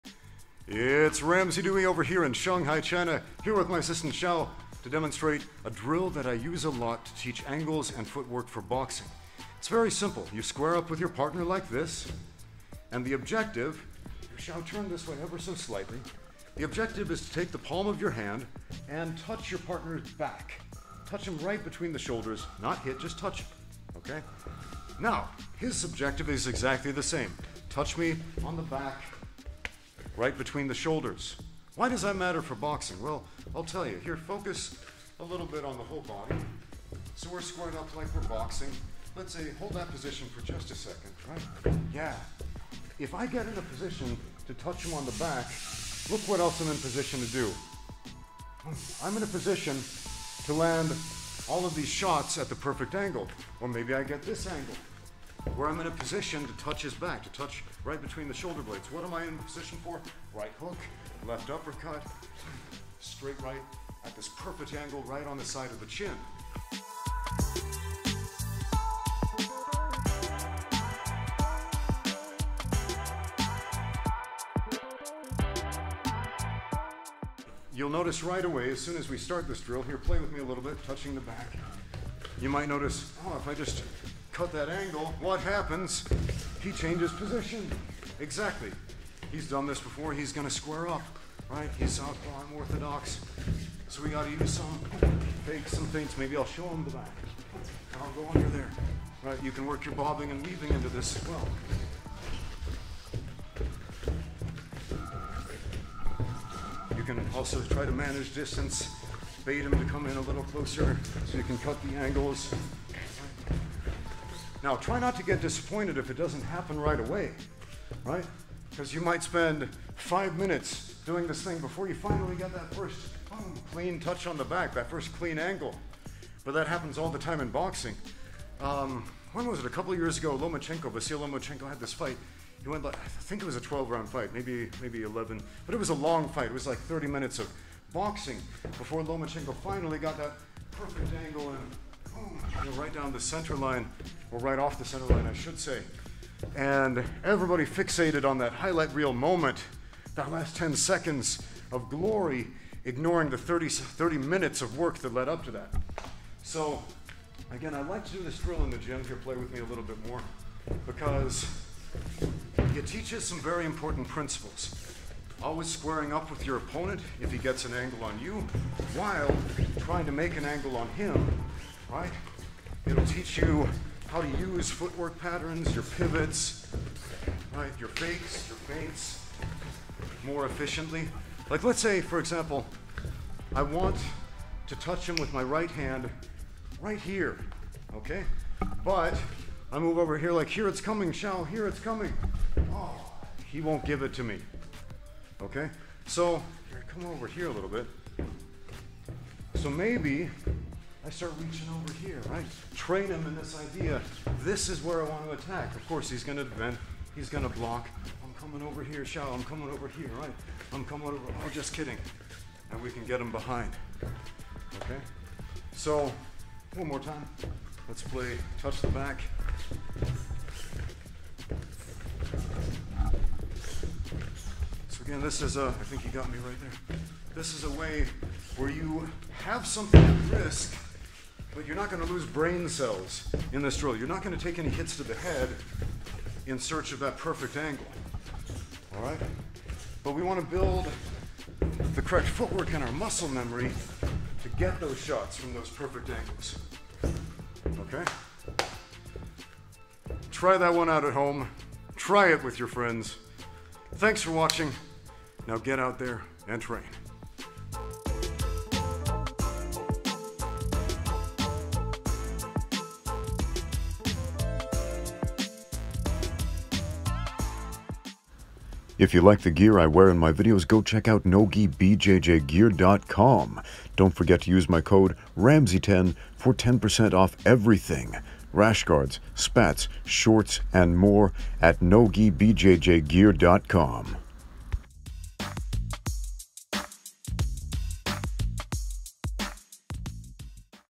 Easy boxing drill to cut angles like a pro
Radically improve your footwork, feints, head movement, and significantly increase your chances of hitting the other guy without getting hit. Try this fundamental boxing positional drill.

Shanghai based MMA Coach and Kunlun Fight Combat League ringside commentator Ramsey Dewey answers questions from the viewers.

This video features original music by Ramsey Dewey